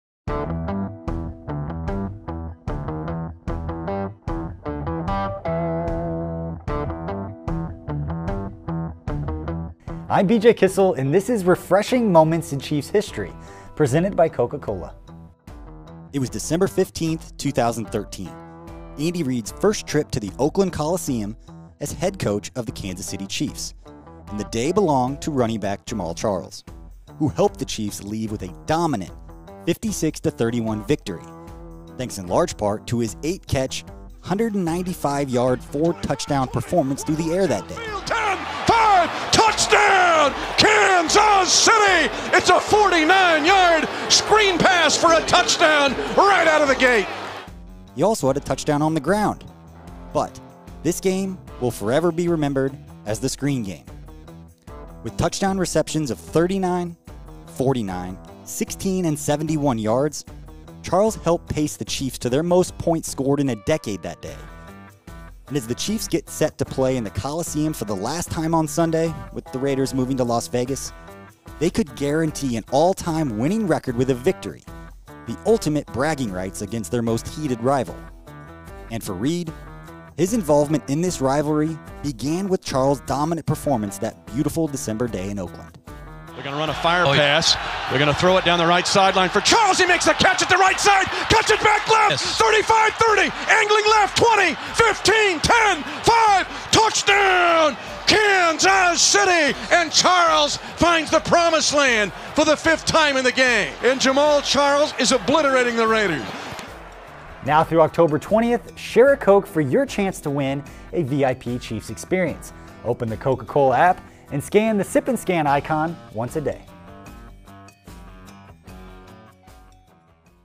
Jamaal Charles Scores 5 TDs vs. Raiders | Refreshing Moments in Chiefs History
Chiefs Reporter BJ Kissel looks back at Jamaal Charles' five touchdown performance against the Raiders in Andy Reid's first game at the Oakland Coliseum as head coach of the Chiefs back in 2013.